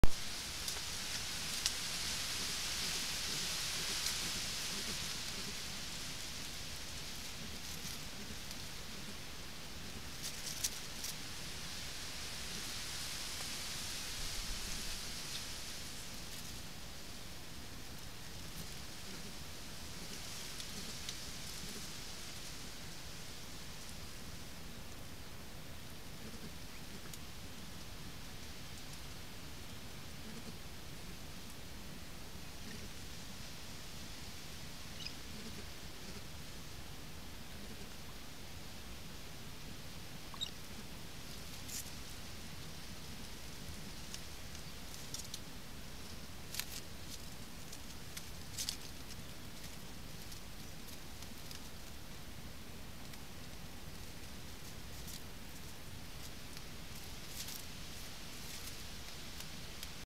Little Bittern (Ixobrychus minutus) - eggs incubation (1)
Little Bittern (Ixobrychus minutus) - eggs incubation, Lubelskie Region, Poland 2013